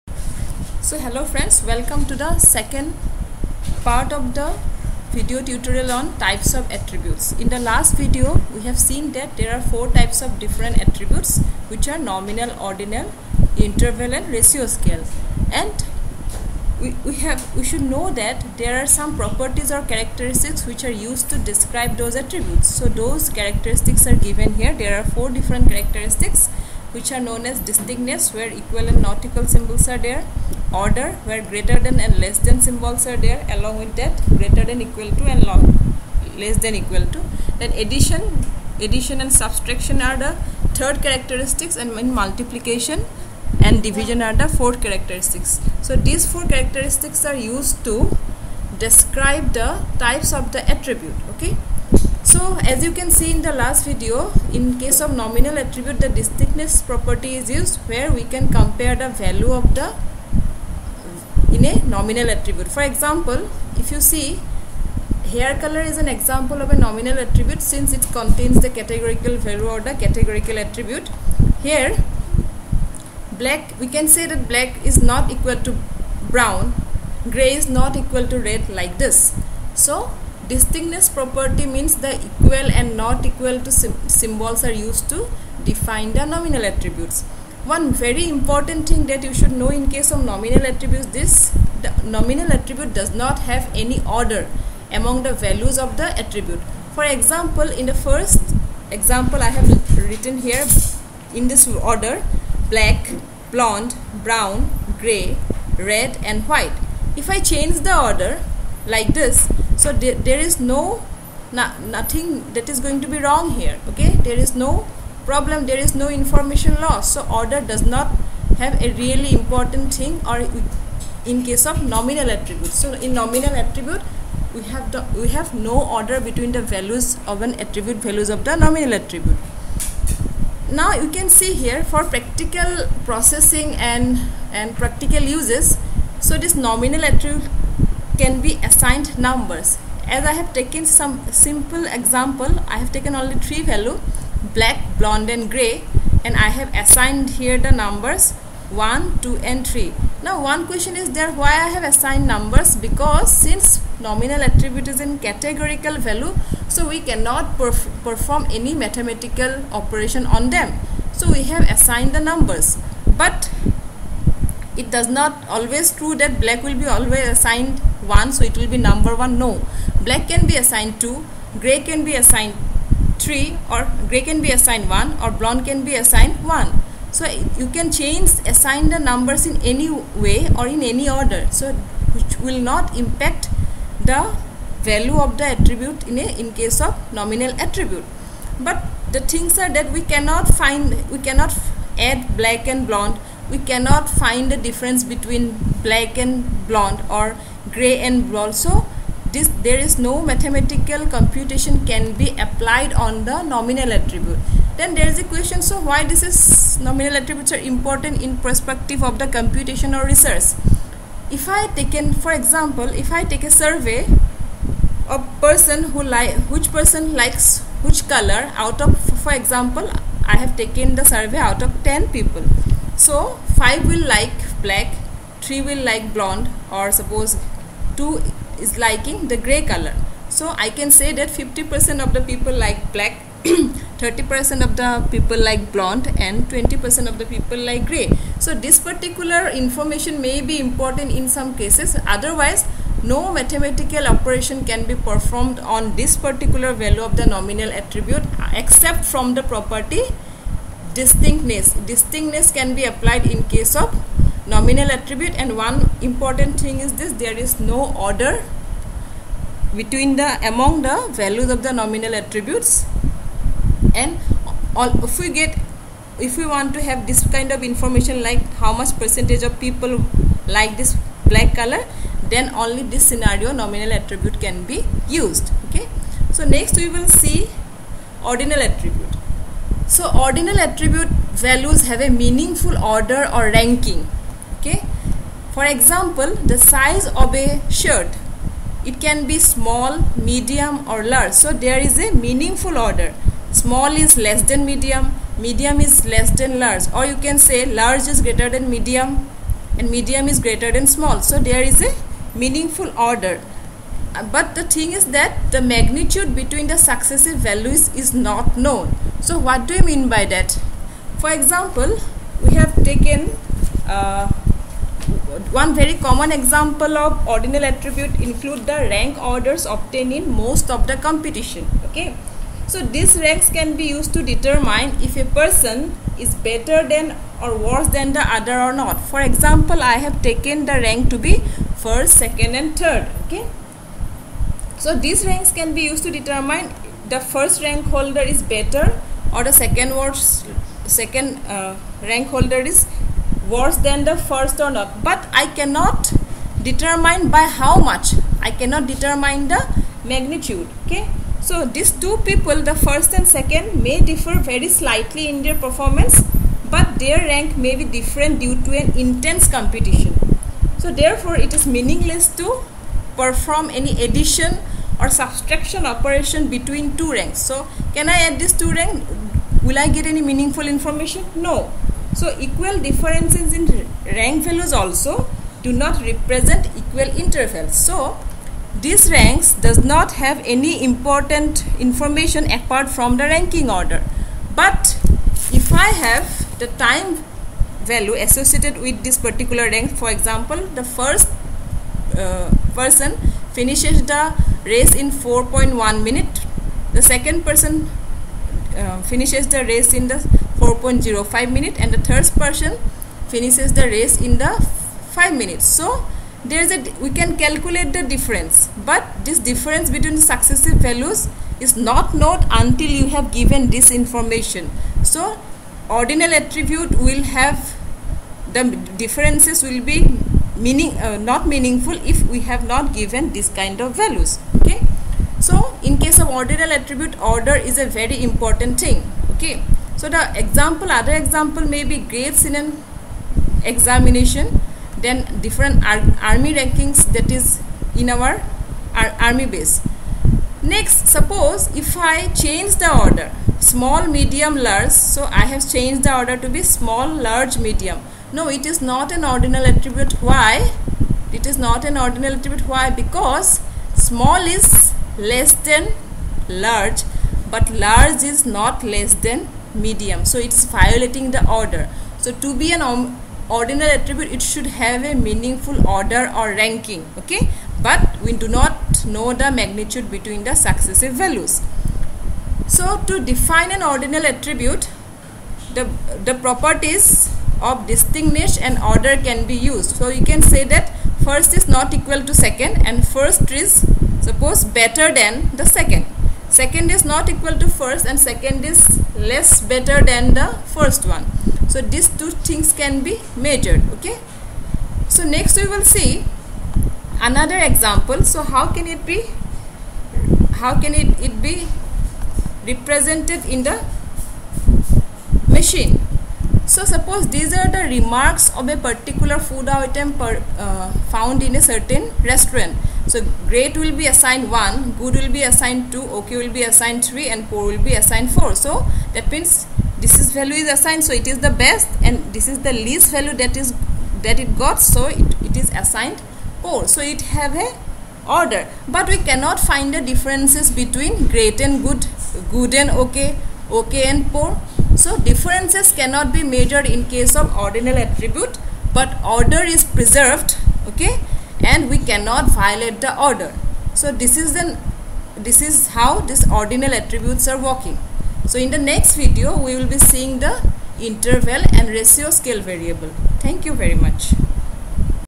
DM6: Types of attributes(part 2)
This video will give you a overview on ordinal attribute